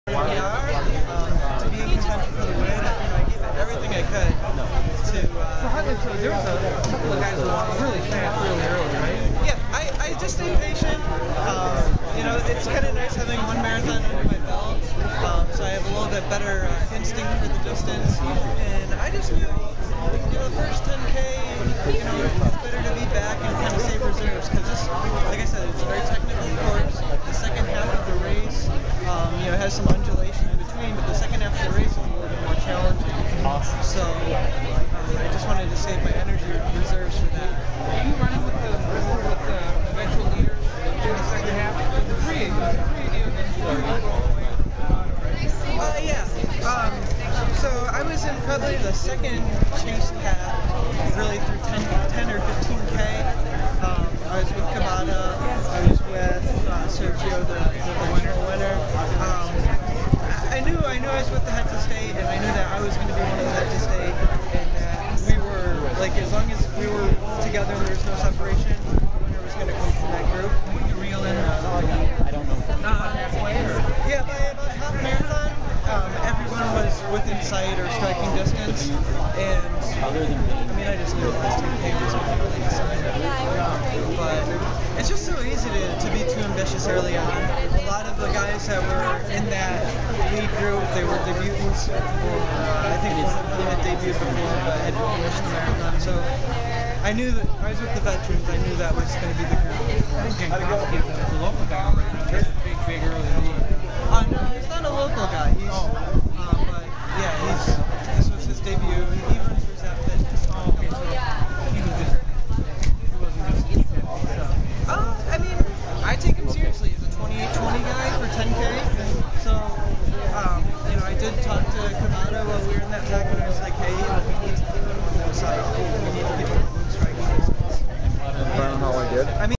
USA Men's Marathon Championship second place winner, Jeffrey Eggleston
Interview with Medtronic Twin Cities Marathon second place winner Jeffrey Eggleston